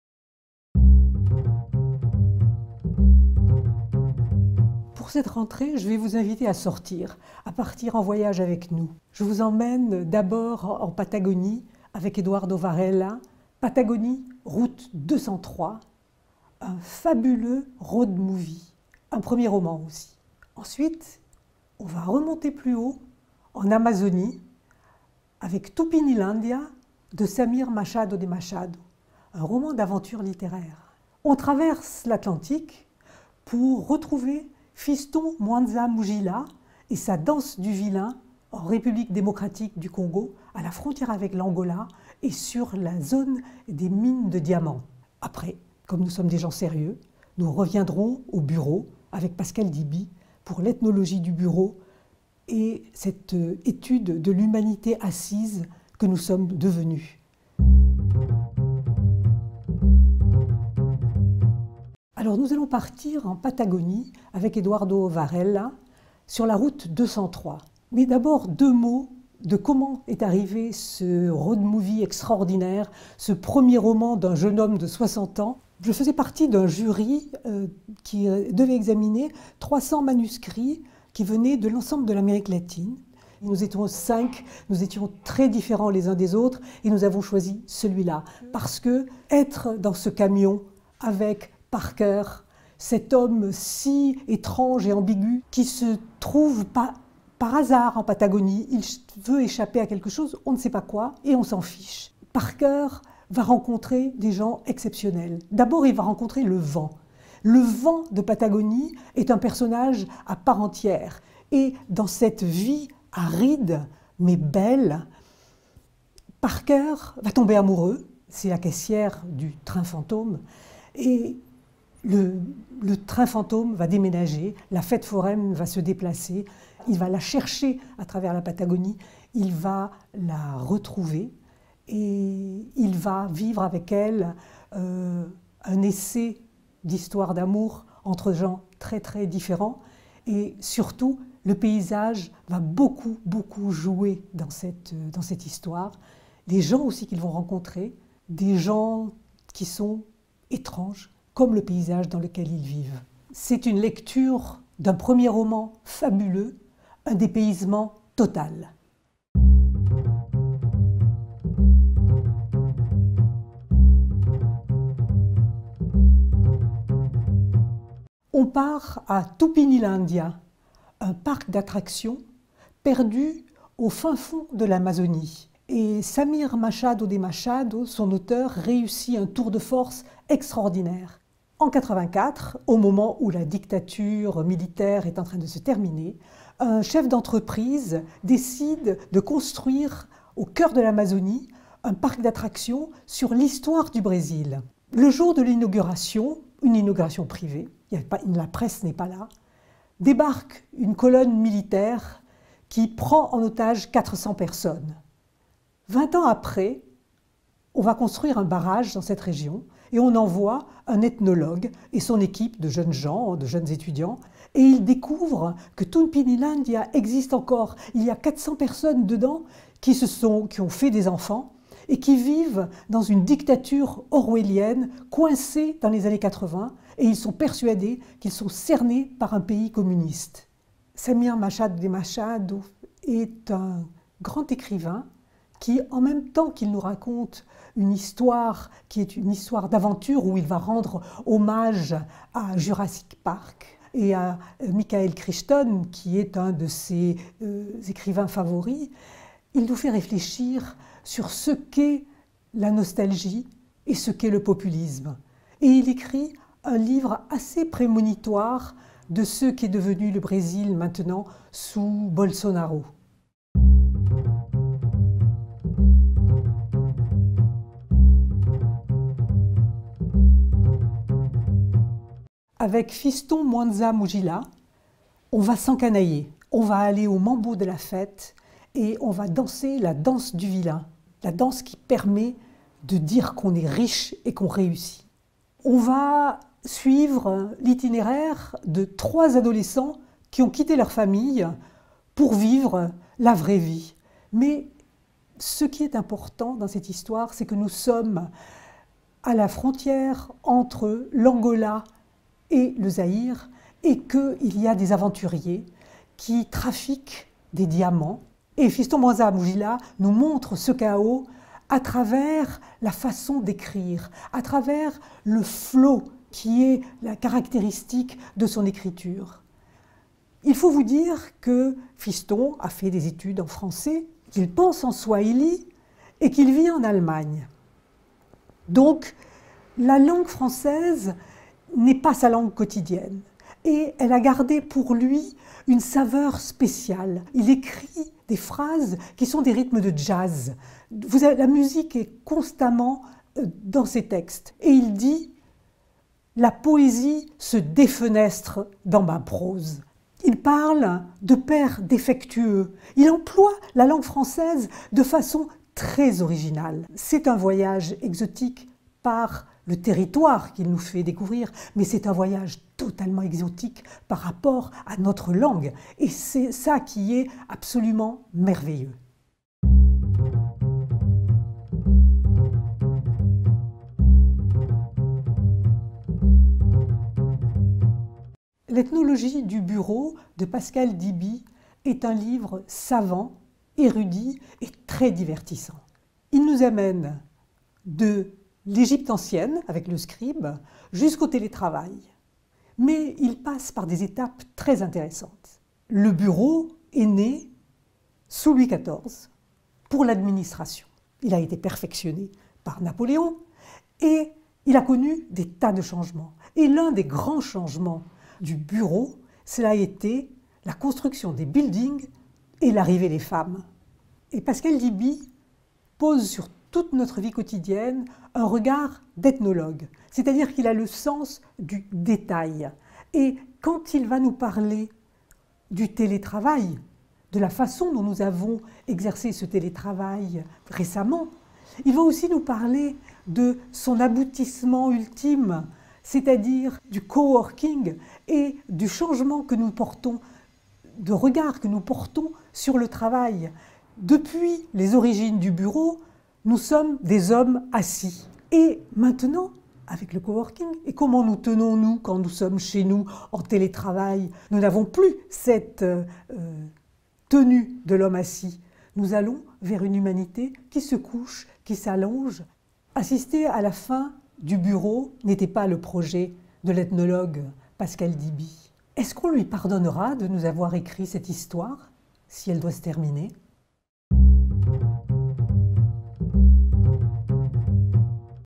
La rentrée littéraire 2020 des Éditions Métailié
Anne Marie Métailié vous présente la rentrée littéraire 2020 des Éditions Métailié !